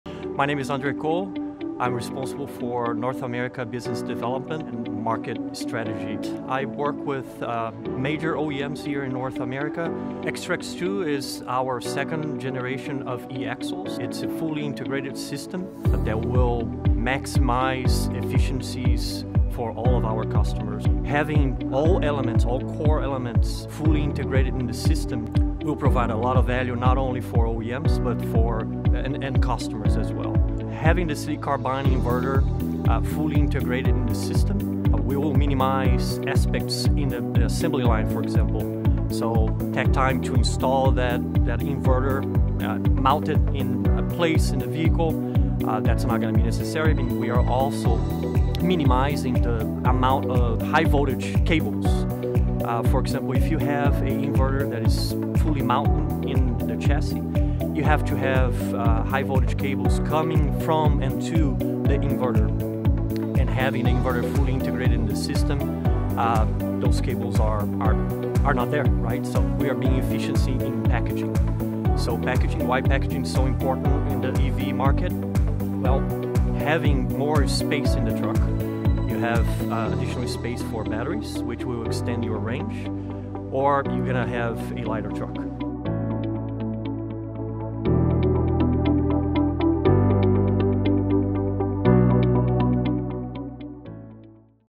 Benefits of ZF AxTrax 2 eAxle
Clean advanced technology is on the rise in commercial transportation. One key player? Electric axles.

Hear how ZF's integrated eAxle, AxTrax 2, is transforming the way vehicles operate and the many benefits it offers fleets adopting electric commercial vehicles.

From simplified maintenance to enhanced efficiency and cleaner operations, AxTrax 2 is making a huge difference.